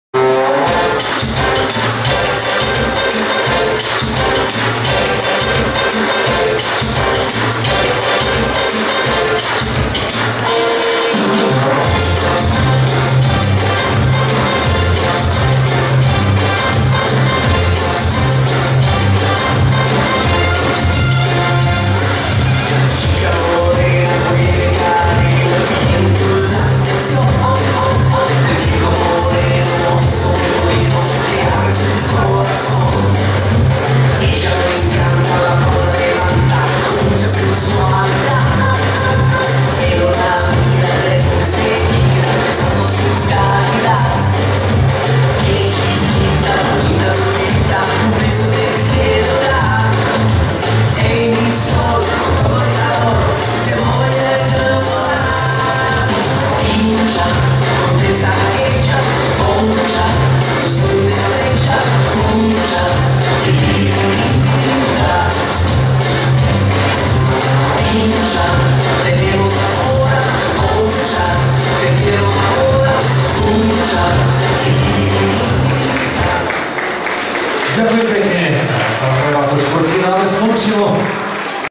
Samko a Lucka tancujú jive na STOP.SHOP DANCE CUP 2011 v Liptovskom Mikuláši